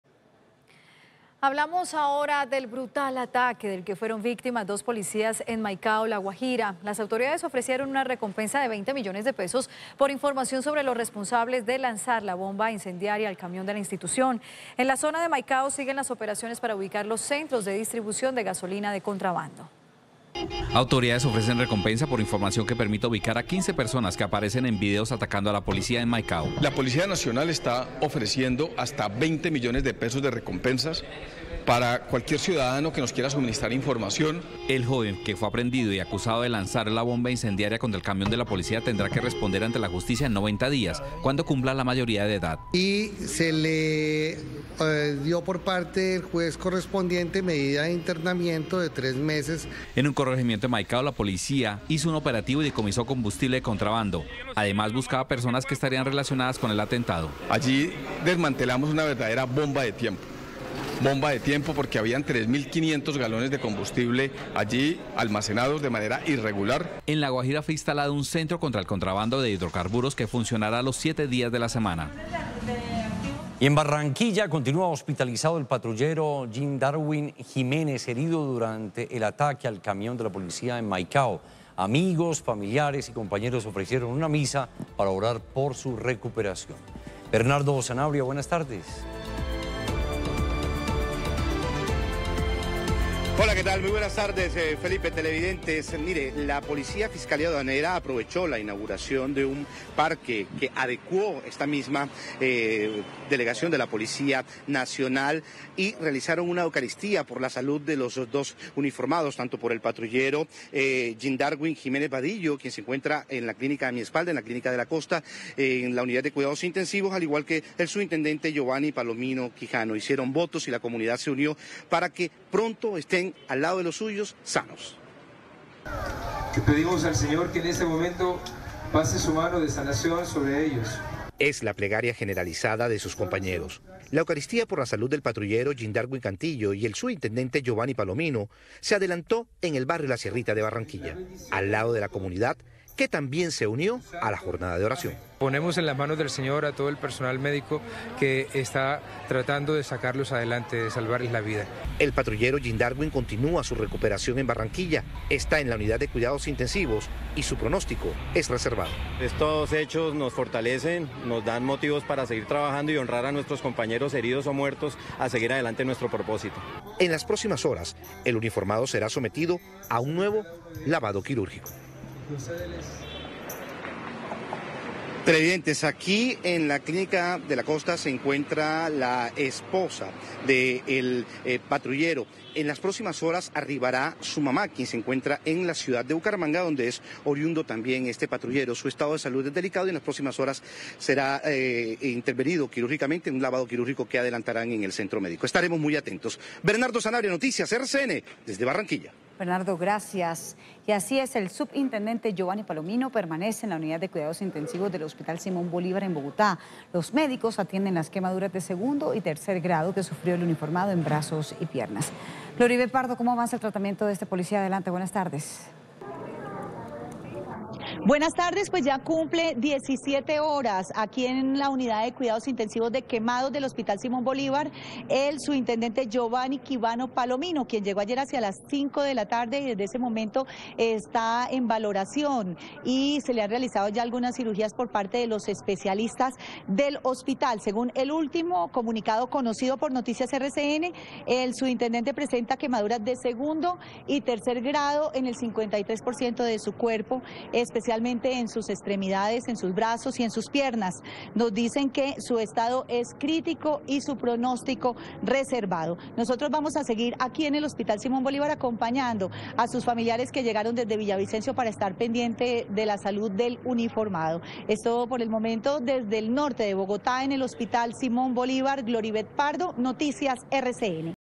$20 millones de recompensa por autores del ataque a policías en Maicao | Noticias RCN
Luego del ataque del que fueron víctimas dos policías, en Maicao siguen las operaciones para ubicar los centros de distribución de gasolina de contrabando ➟